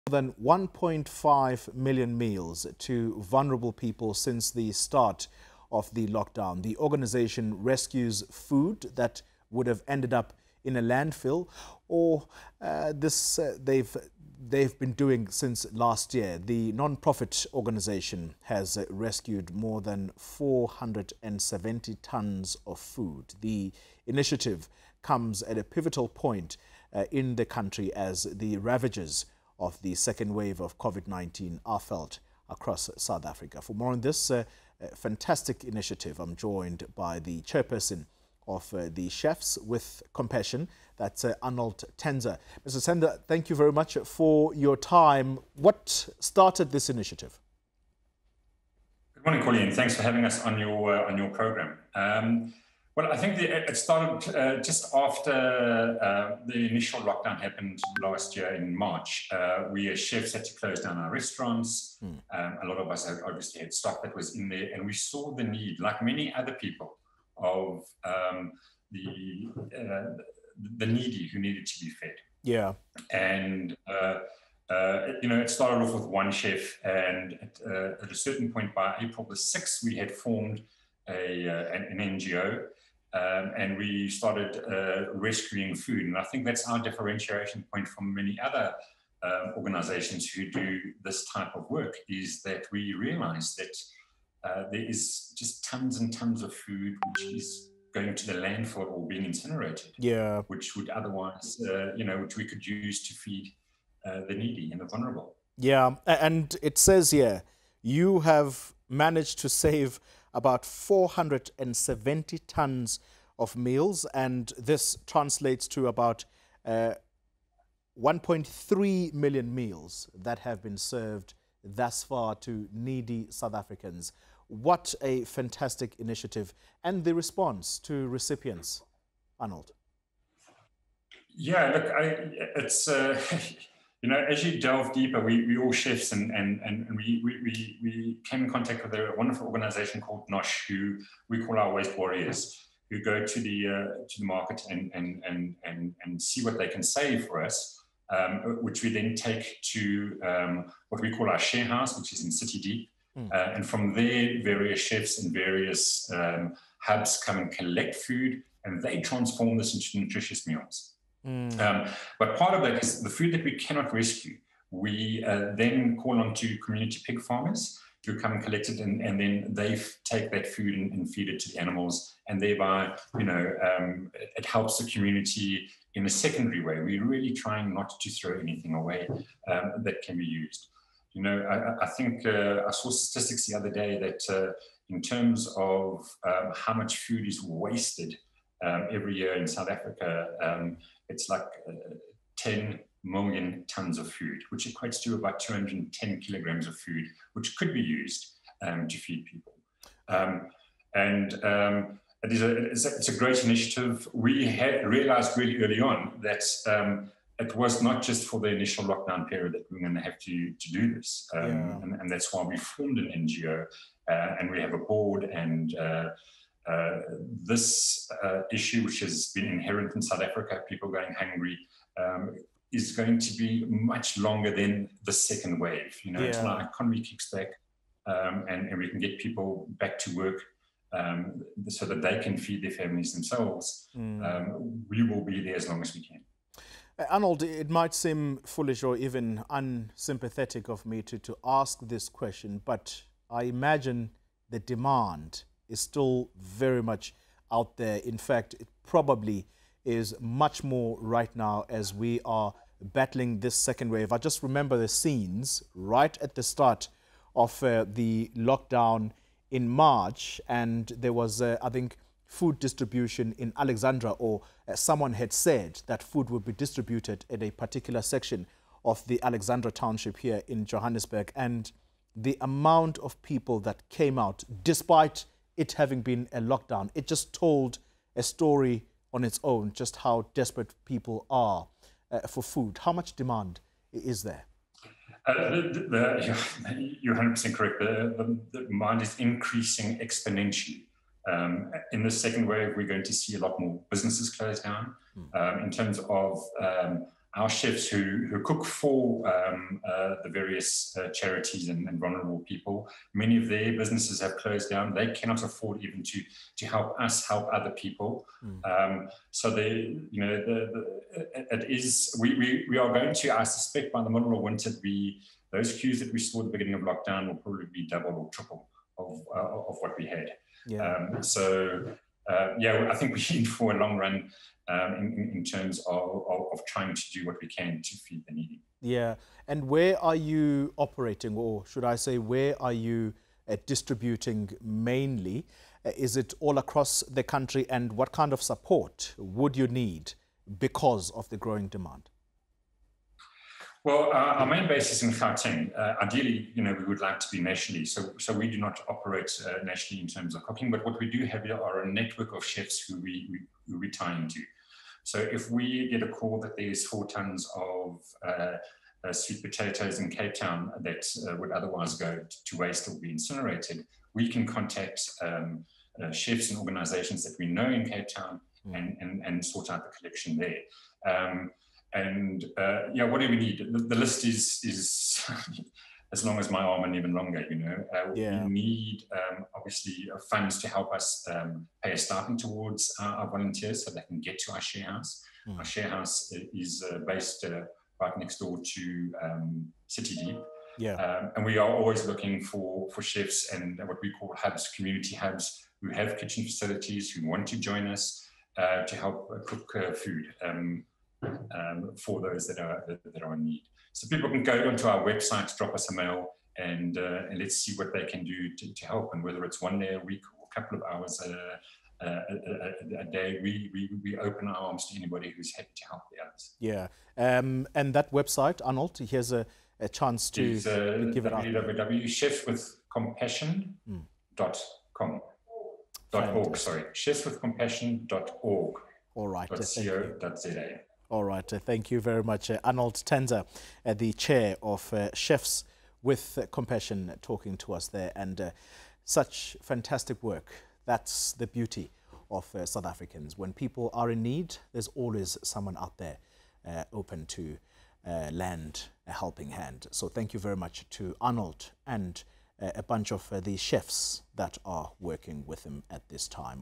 Chefs turn 'waste food' into 1.3 million meals for the hungry
A gaggle of native chefs have turned meals that might have gone to waste into greater than 1.3 million meals for individuals battling by means of the Covid-19 pandemic. Arnold Tanzer, Chair of Chefs with Compassion has more on the initiative.